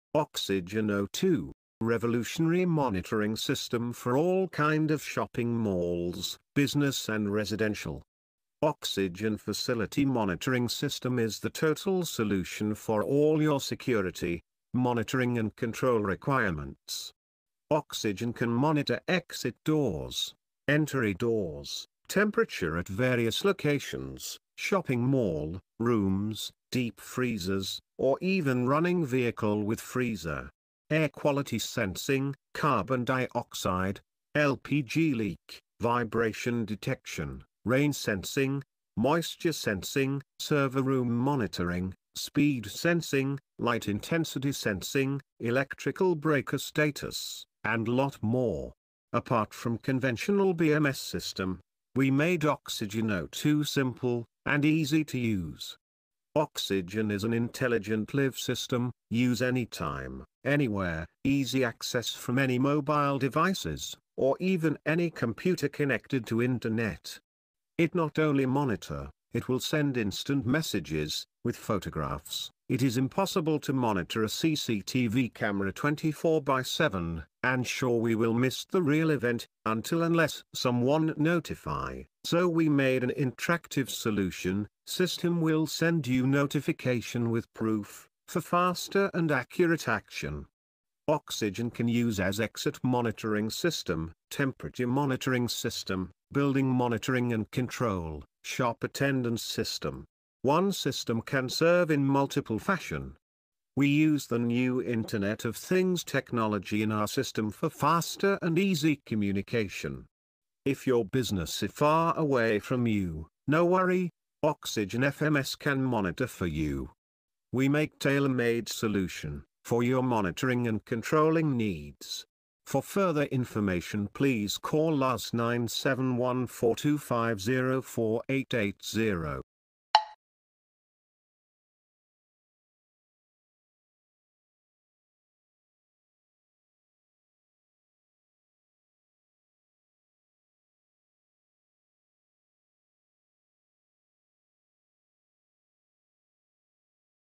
Oxygen O2 Facility Monitoring system - Monitor & Control anything from anywhere.
Internet of Things  - Facility Monitoring system
Oxygen  O2 can monitor Exit doors, LPG leak, Co2, Air quality, Air dust content, Temperature, Ultra low temperature monitoring, Light intensity, human presence, Movement detection, Thermal detectors. 24x7 full speed scanning 2000 times in a second, accurate reading and high reliability. Live push notifications - Live messenger integration with live  Internet Of things Oxygen O2
Leaders Line Trading LLC
Oxygen o2, IoT dubai
Light weight communication protocol for fast and easy notification. Few bytes can do the job. Exit door monitoring to the new level of security and protection. System can send live photos if any events happened. Live monitoring can be installed in any number of devices. Mobile device support, Can easily monitor from your convenience. Very low data consumption

Oxygen compatible with all kind of electrical accessories.